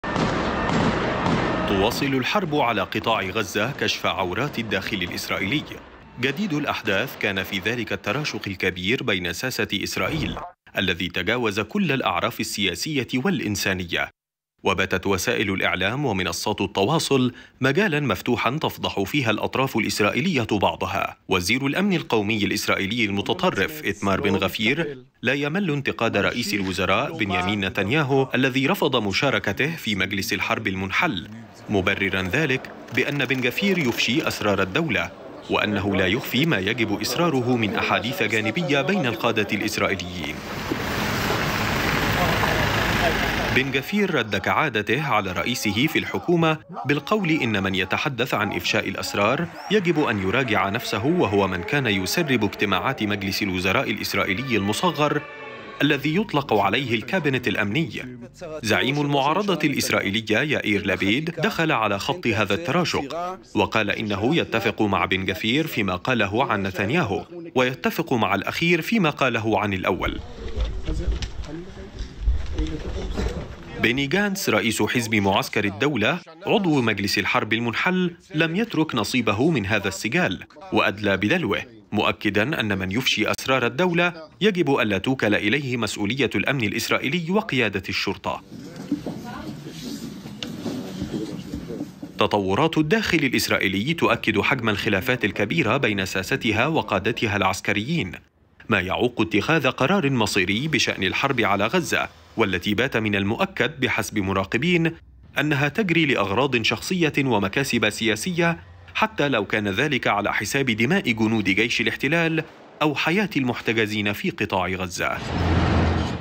شقاق كبير بين الساسة والقادة العسكريين .. تراشق سياسي تجاوز كل الأعراف في إسرائيل
20/6/2024